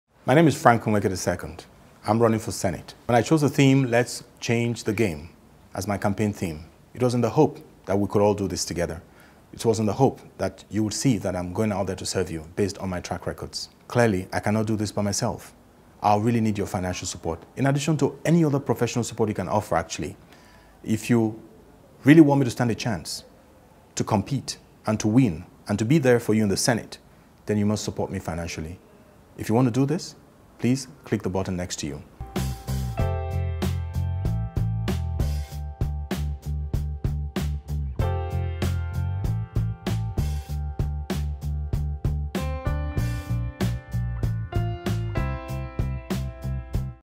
Frank Nweke is Betting on You
I'm of the opinion that in order to bring fundamental change government one must have the courage to look in a new direction - which could sometimes be a lonely path. If I'm to make it into the Senate, I want to be beholden only to the voters who put me there, and not to any godfather.  If this message resonates with you and you think that we can truly transform governance one leader at a time then support me by going and make a contribution to my campaign.